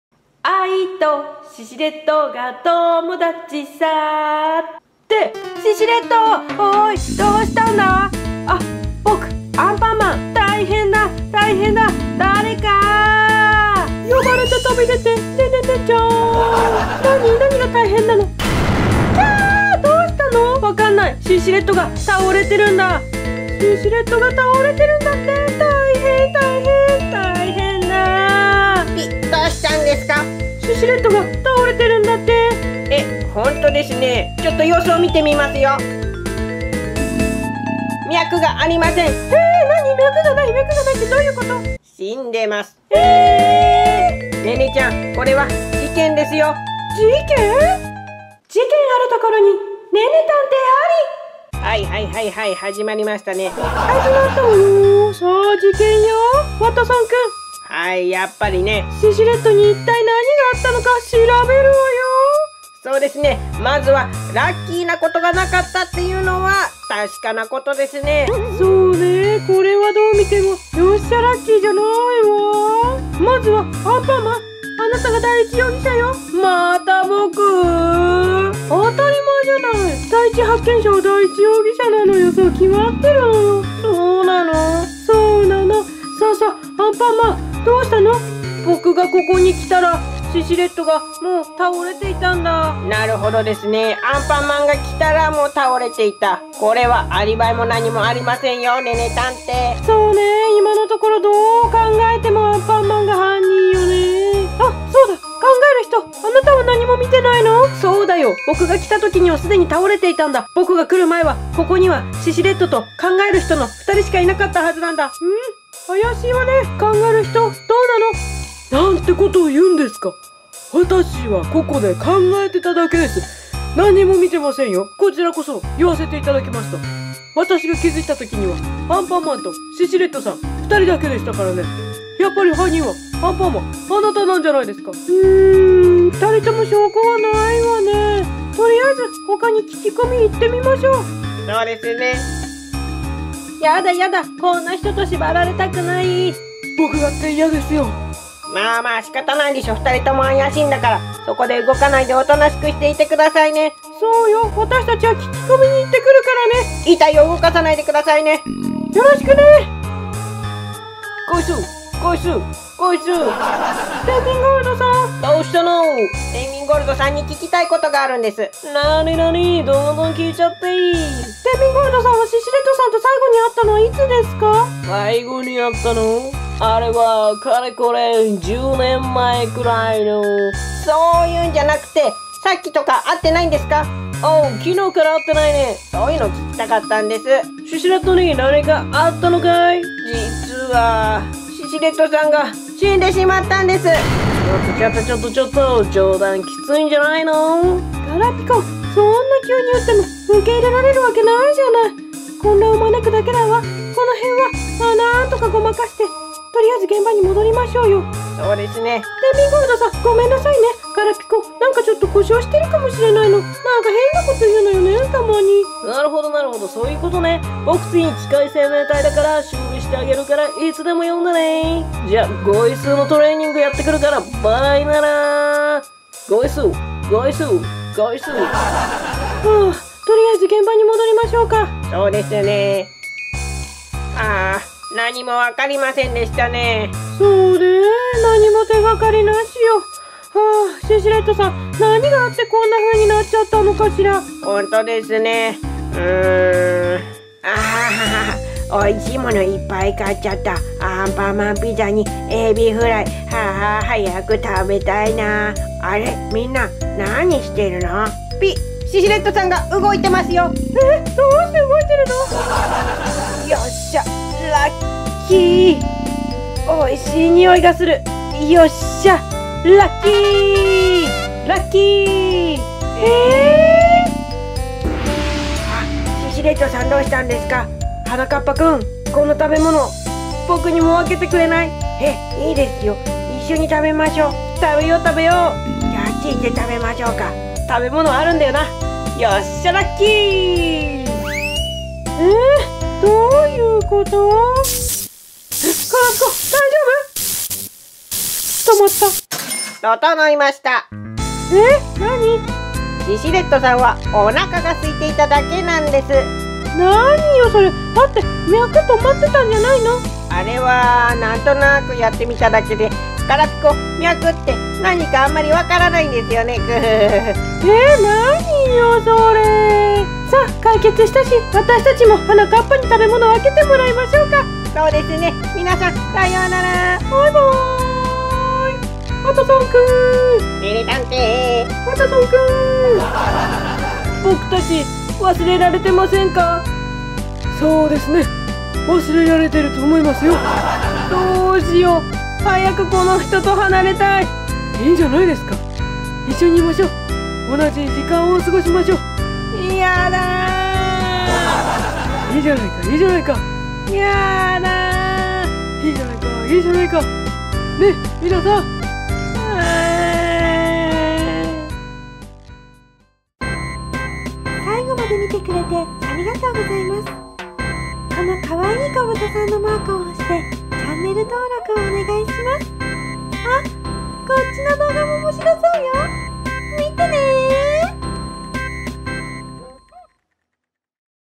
ネネたんていのじけんぼ。その5～シシレッドが倒れた！ こどモす おもちゃアニメ シシレッド ネネちゃん ガラピコ kyuranger
シシレッドがたおれてる！？
なぜ倒れたのか。そして、シシレッドは一体どうなってしまうのか！？
これは大事件！
そして大事件と言えばこの人！ネネ探偵。
今回も事件解決となるのか！？